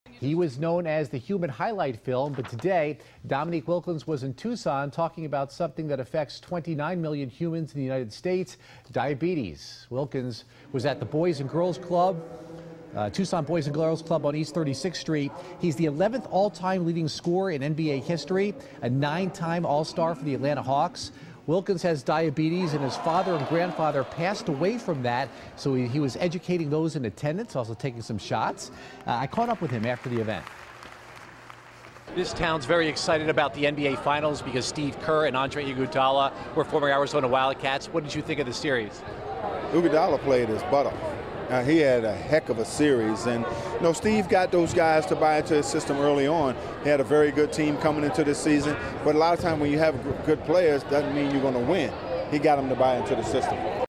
NBA Hall of Famer Dominique Wilkins in Tucson
The nine-time all star visited the Tucson Boys and Girls Club to raise awareness for diabetes